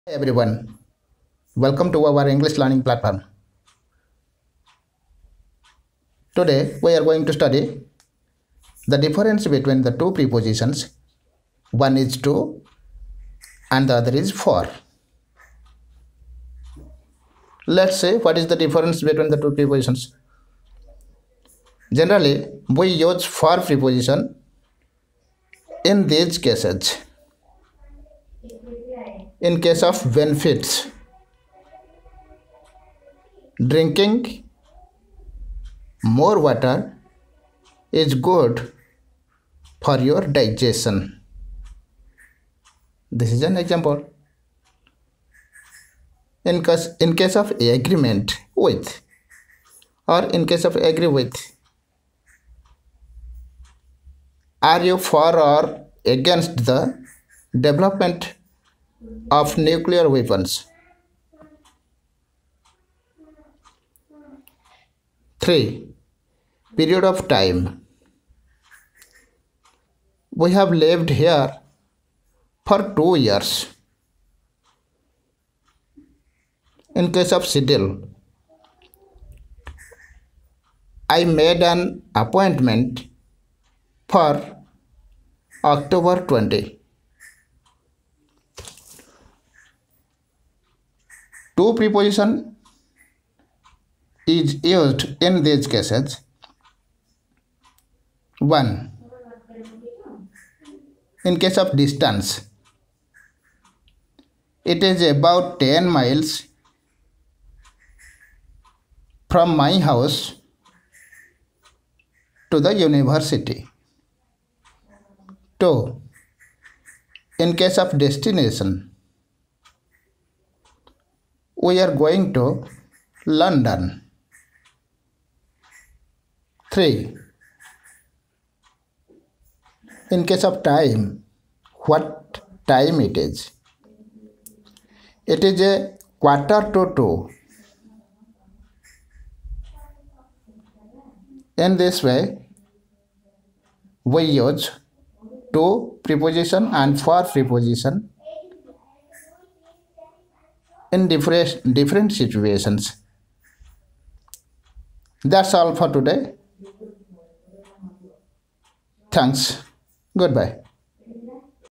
DIFFERENCE BETWEEN 'TO' AND 'FOR' PREPOSITIONS || ENGLISH GRAMMAR || GURUJI PRAFUL
The difference between the two prepositions 'to' and 'for' has been described here in this video. It may help the beginners to learn English easily.